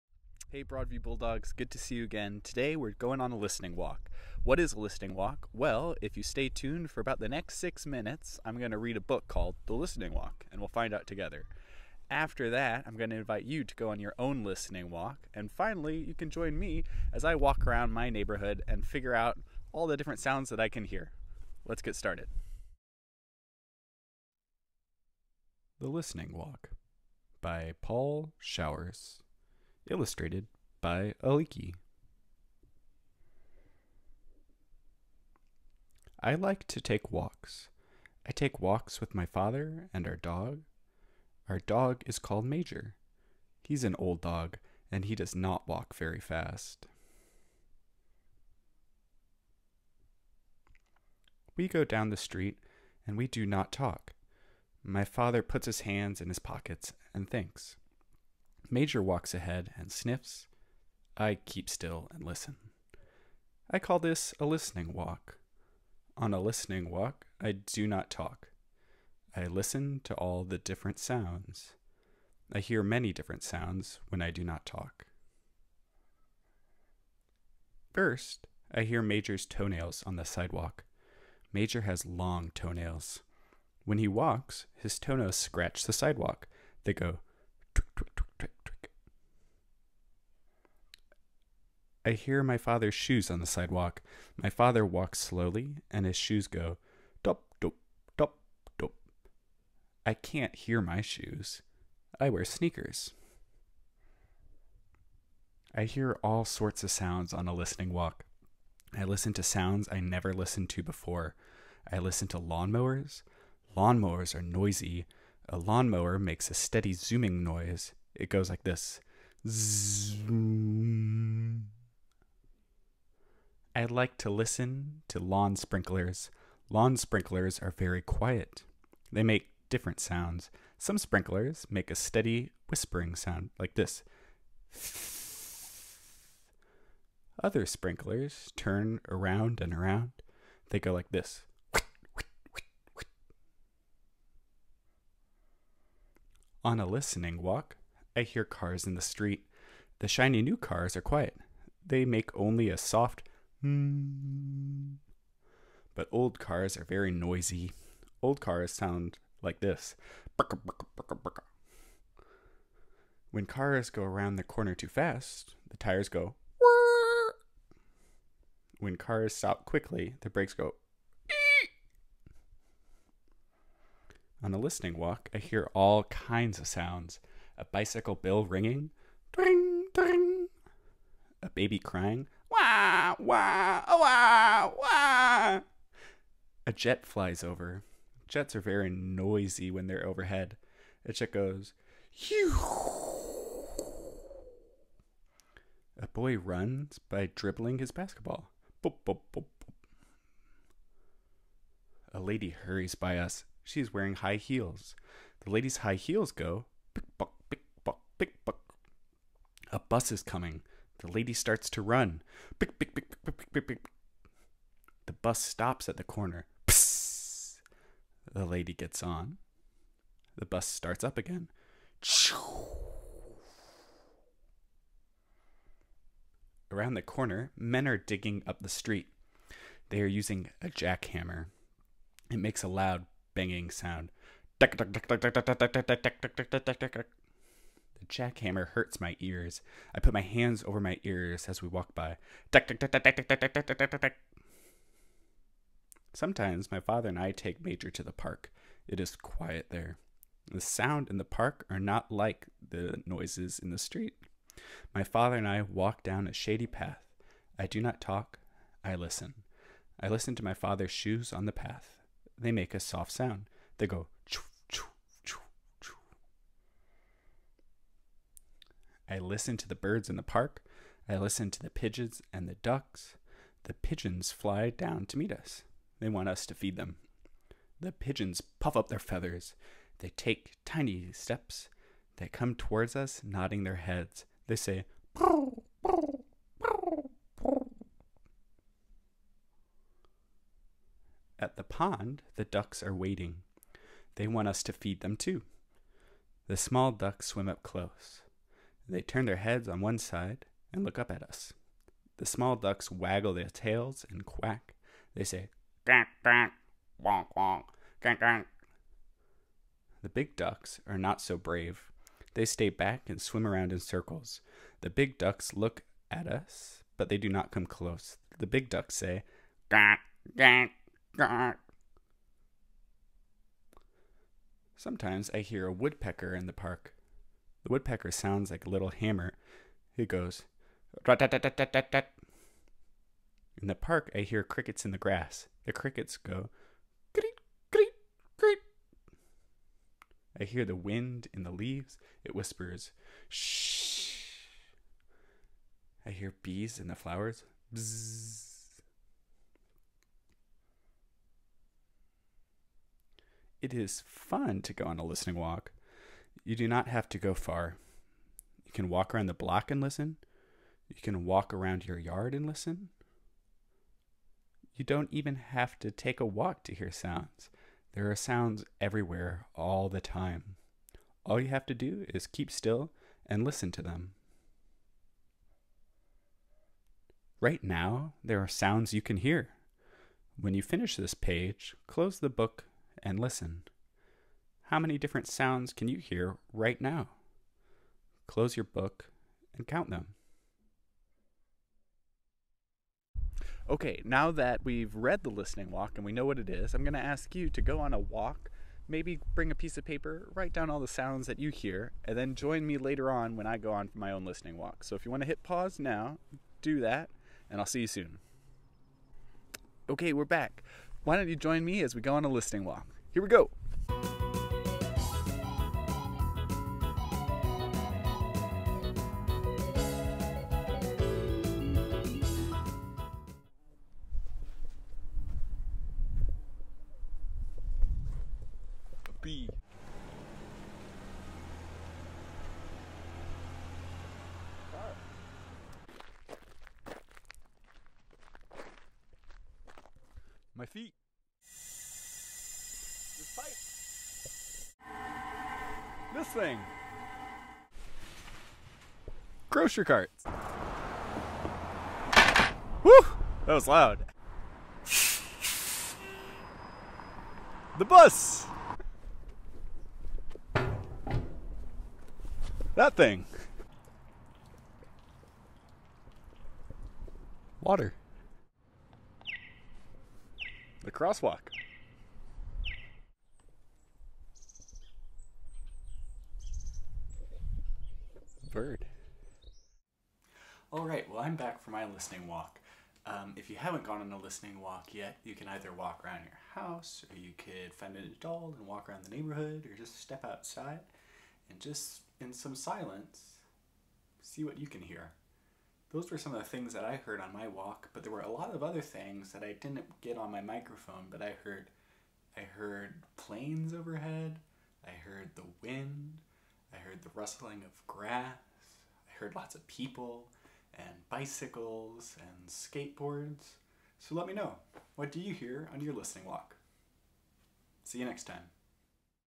Listening Walk
A read-aloud of the book, "The Listening Walk";  an at-home activity, wherein you can keep track of all the things you hear around your house or neighborhood; and join me again at the end of the video to tag along on a listening walk with Mr. Dahm.